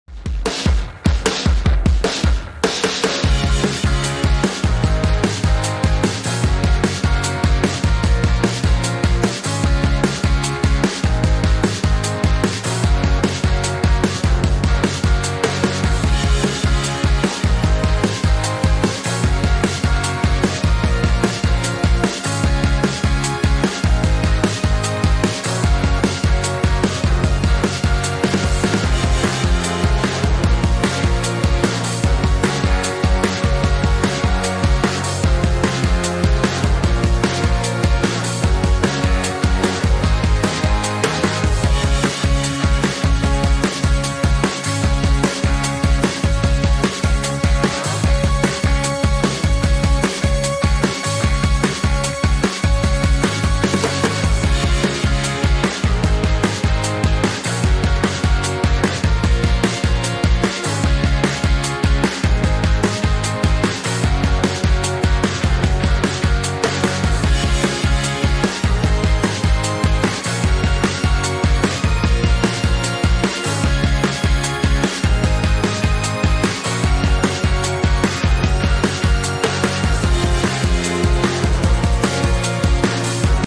TSA REEL 1
Reliving the magic on the field! ⚽✨
From game-changing goals to epic tackles and unstoppable saves – these moments define our passion and drive! 💥🔥 Witness the energy, skill, and teamwork that make every match unforgettable. 🏆💪 Ready for more? Let's keep pushing forward! 💯